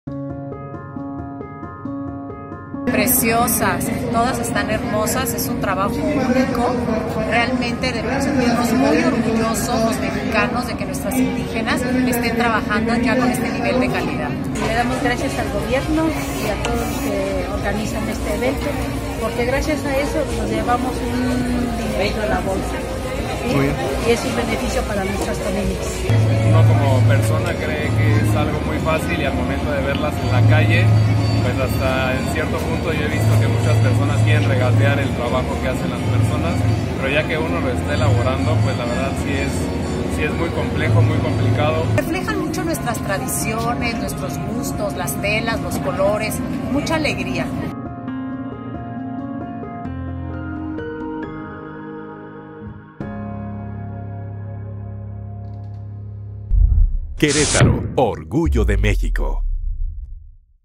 ¡Lele regresó a QroOrgulloDeMx!
El "18° Encuentro de las Culturas Populares y los Pueblos Indígenas en Querétaro" concluyó con el esperado regreso de Lele a Querétaro, la ocasión ideal para conocer el talento y el trabajo de nuestros indígenas, por los que somos QroOrgulloDeMx.